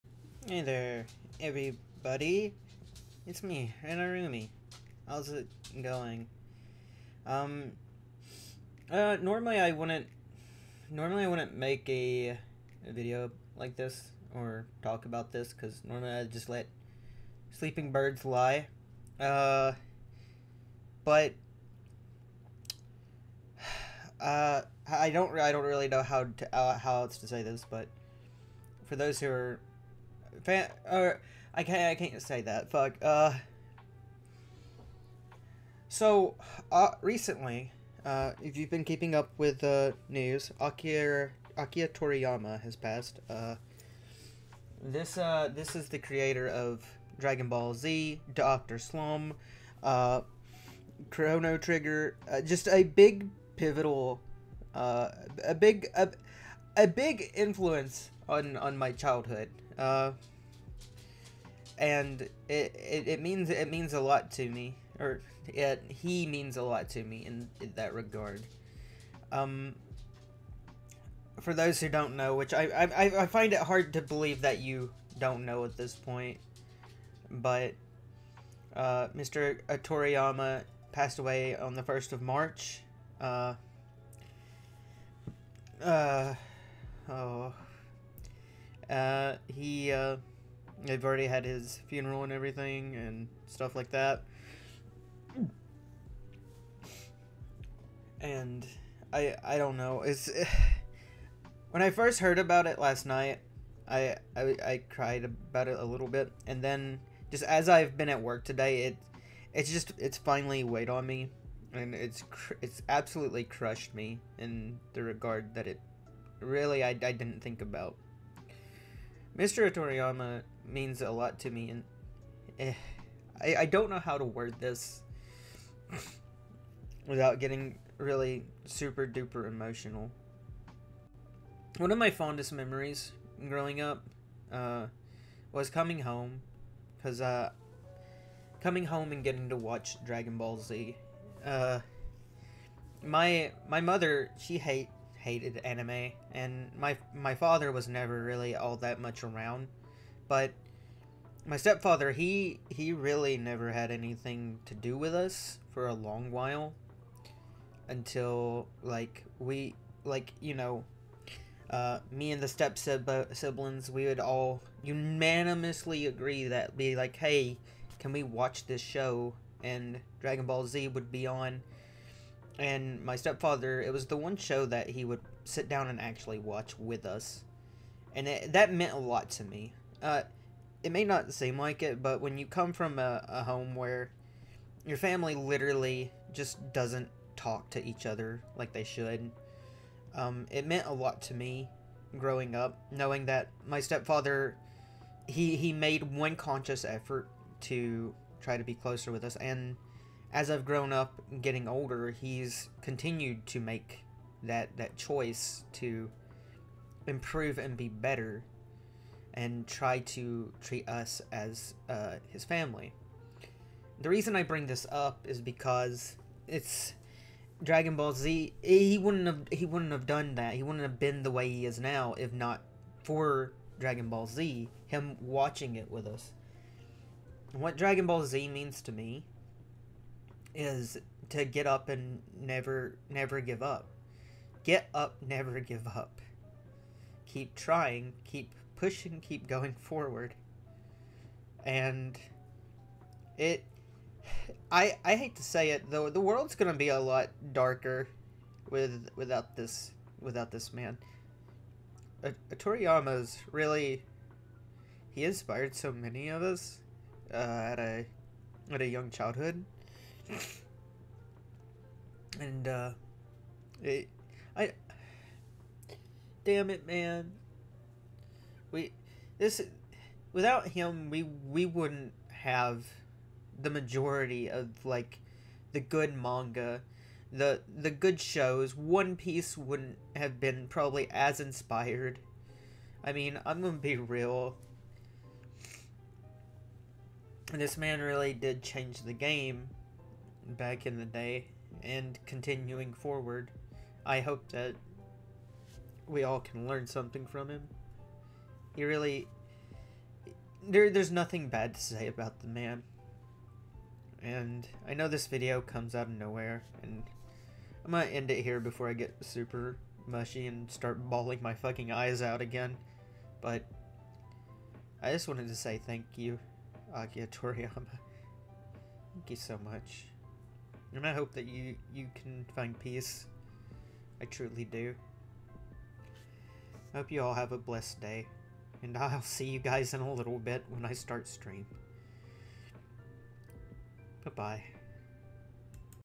Thank you Akira Toriyama
Sorry if I cry this man shaped my childhood for the better could not sit and not say goodbye.